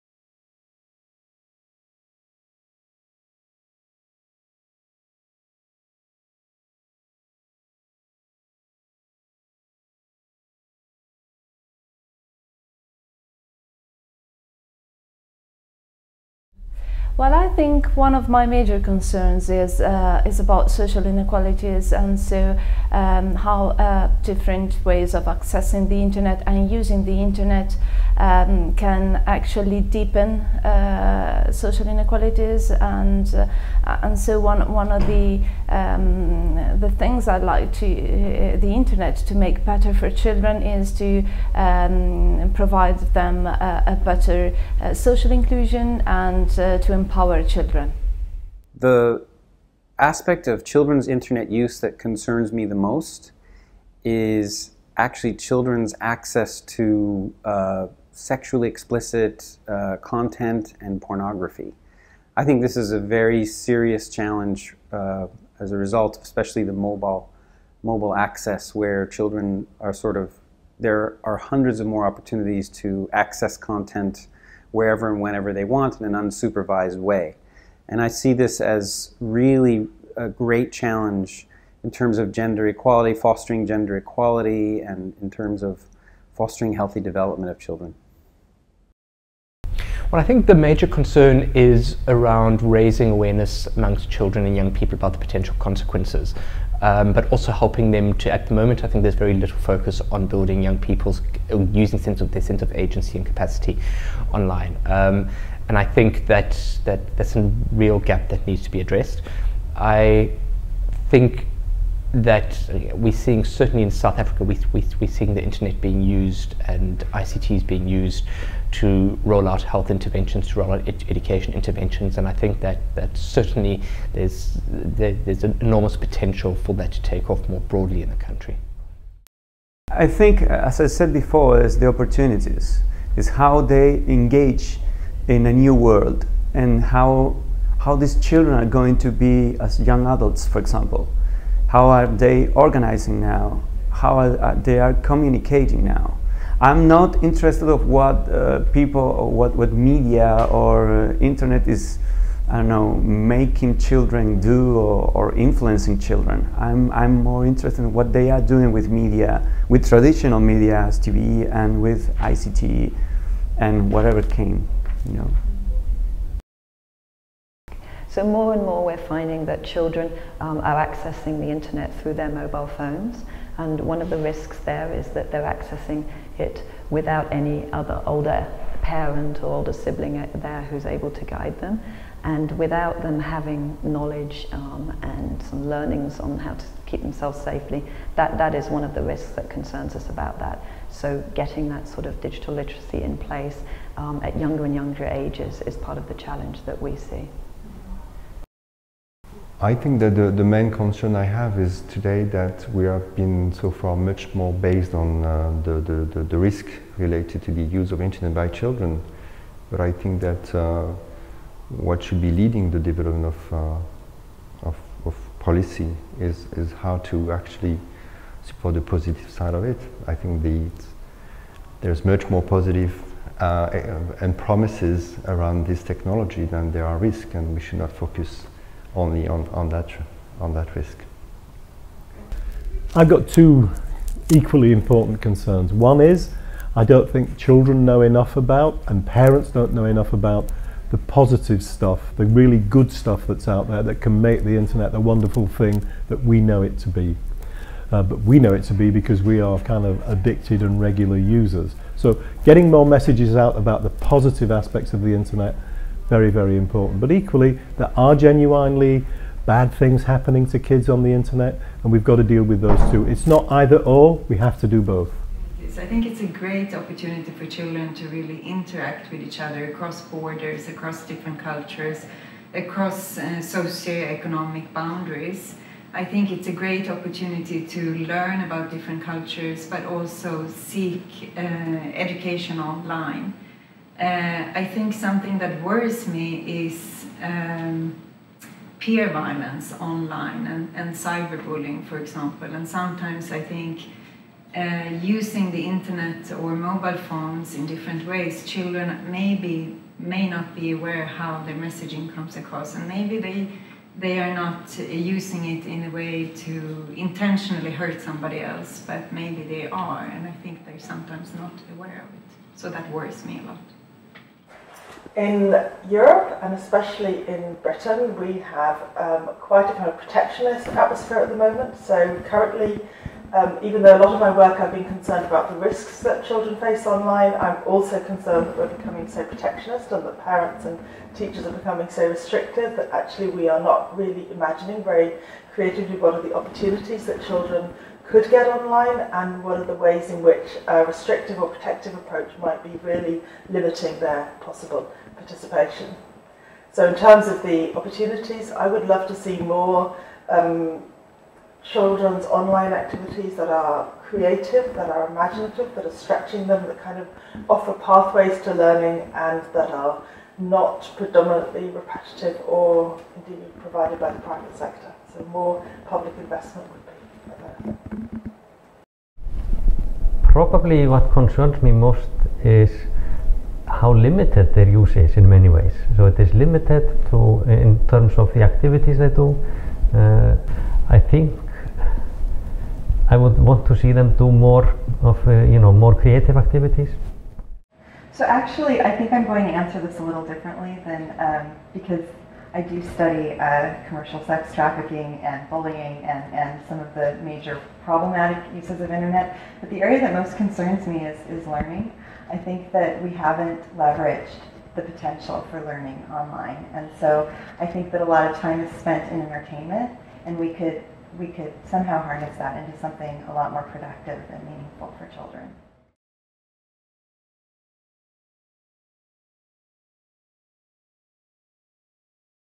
Can the internet make a better contribution to children’s opportunities and well-being?
International experts in the area of child rights, child protection, internet and mobile technologies governance and applied and policy-relevant research discuss what concerns them the most about children’s internet use and express their views on the areas of the internet’s biggest contribution.

The experts were brought together at the meeting organized by UNICEF and the EU Kids Online research network at London School of Economics in February 2015. The aim of the meeting was to evaluate current understandings of the risks and opportunities afforded to children worldwide as they gain access to internet enabled technologies, and to explore the feasibility of developing a global research framework to examine these issues further.

Contributors -
1. Giovanna Mascheroni – Catholic University of Milan, Net Children Go Mobile
2. Dale Rutstein – UNICEF Office of Research-Innocenti
3. Patrick Burton – Centre for Justice and Crime Prevention (CJCP)
4. Patricio Cabello – Pontificia Universidad Católica de Valparaíso
5. Natasha Jackson – GSMA
6. Yves Boillot – Orange
7. John Carr – ECPAT International
8. Clara Sommarin – UNICEF
9. Sonia Livingstone – London School of Economics (LSE), EU Kids Online
10. Kjartan Ólafsson – EU Kids Online, Health Behaviour in School-aged Children (no sound!)
11. Monica Bulger – Berkman Center for Internet & Society